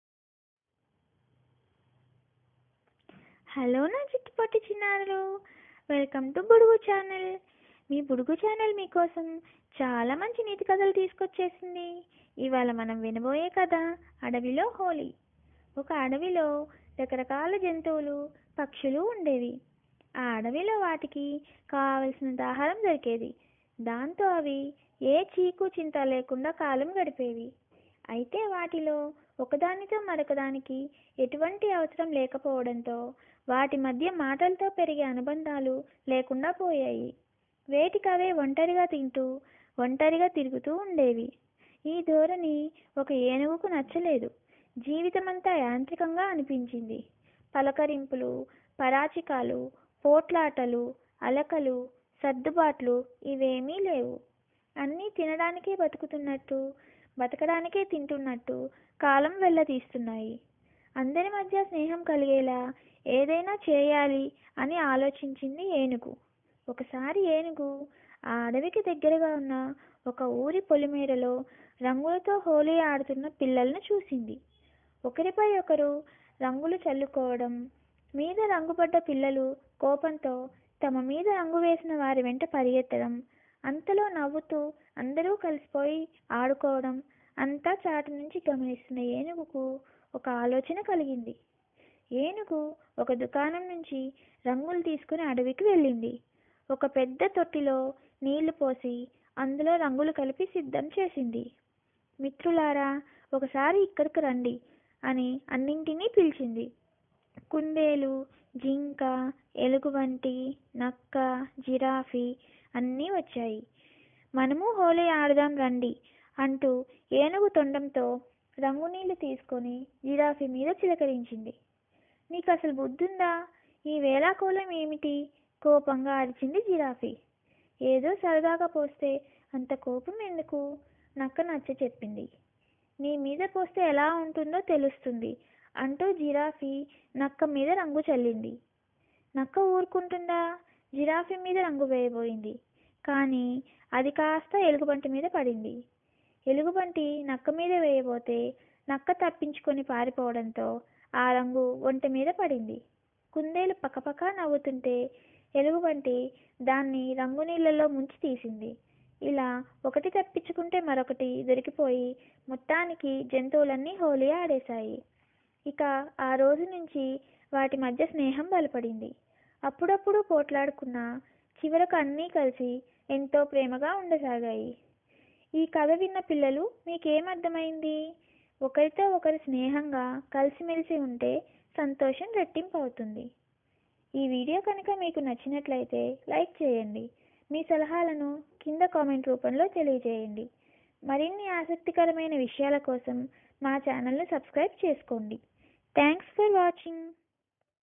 Adavi lo HOLI
Chinna pillalaku nethi kathalu, ee roju katha Adavi lo Holi.